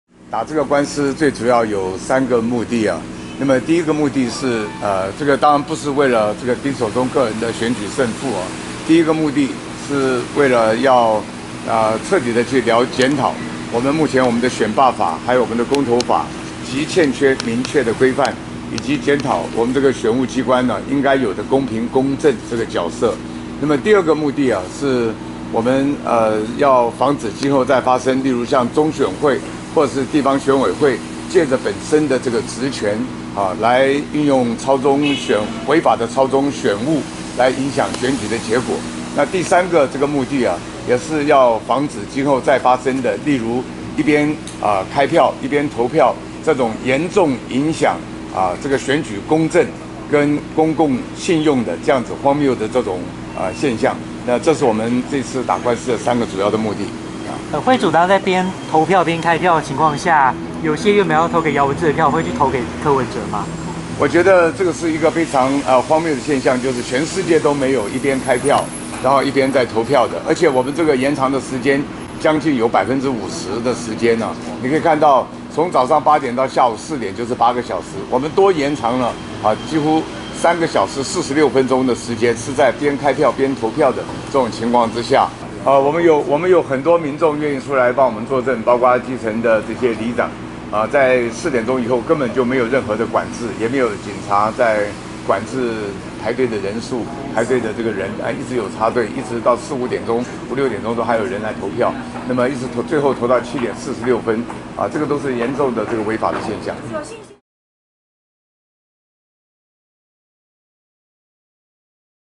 北市長選舉無效首開庭 丁守中：不是為了個人勝負
今年台北市長選舉丁守中小輸柯文哲3254票，丁聲請台北地院驗票，結果還多輸313票，進而提起選舉無效之訴。台北地院今首開庭，丁守中庭前表示，當天投票時間多了將近一半，投票率卻只有65%，顯示很多人都沒辦法投票，「我們一切交給司法，有信心會還給我們公道」。